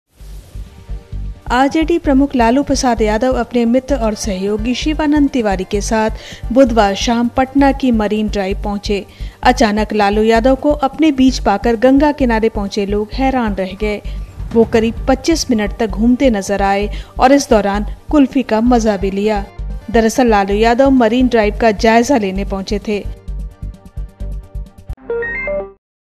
शिवानंद तिवारी के साथ लालू पहुंचे पटना मरीन ड्राइव | Patna Marine Drive
Lalu Yadav: आरजेडी प्रमुख लालू प्रसाद यादव अपने मित्र शिवानंद तिवारी के साथ पटना मरीन ड्राइव पहुंचे और कुल्फी का मजा लिया